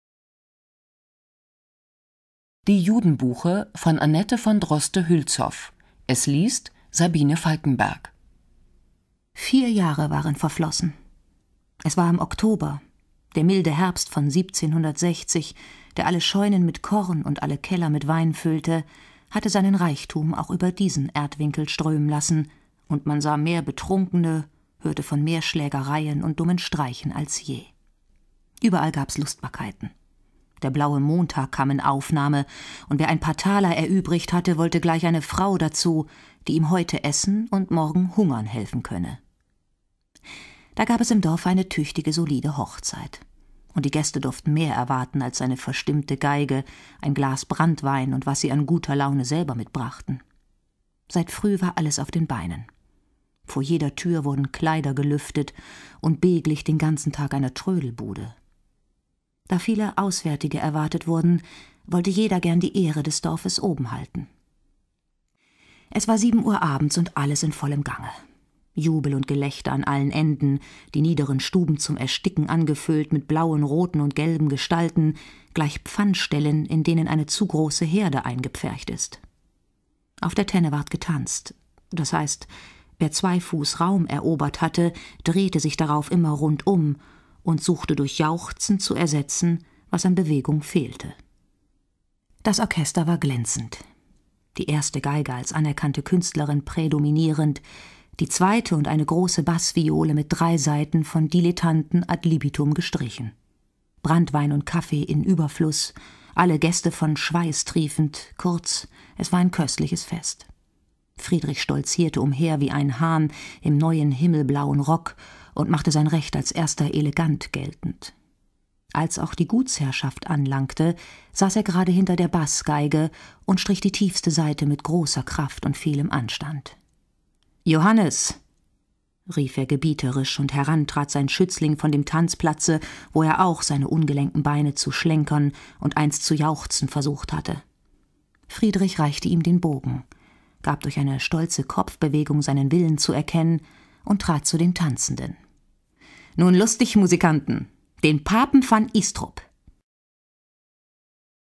Die Judenbuche - Annette von Droste-Hülshoff.wmv
Die "Judenbuche" ist Annette von Droste-Hülshoffs berühmteste Erzählung. Noch heute kennt sie jedes Kind Westfalens. Der Stoff geht auf einen Mordfall zurück, der sich im Gutsbezirk ihre Großvaters Werner Adolf von Haxthausen zutrug. Aus der Reihe HörGut! Klassik - jetzt auch als Download.